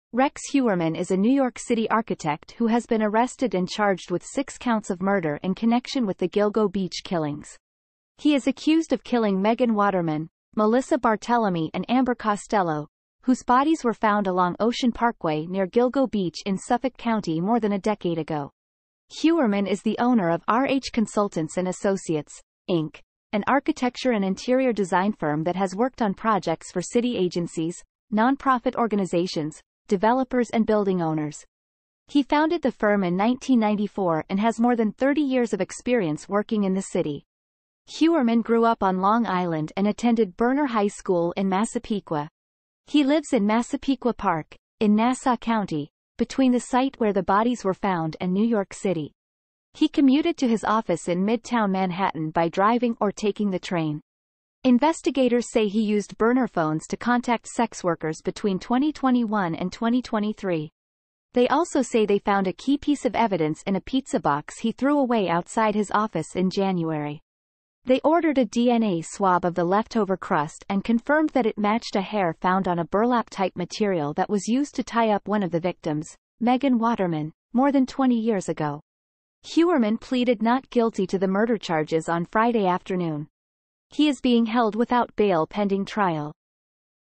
long island serial killer | Rex Heuermann
gilgo beach serial killer suspect heuermann arrested for 3 first degree murders.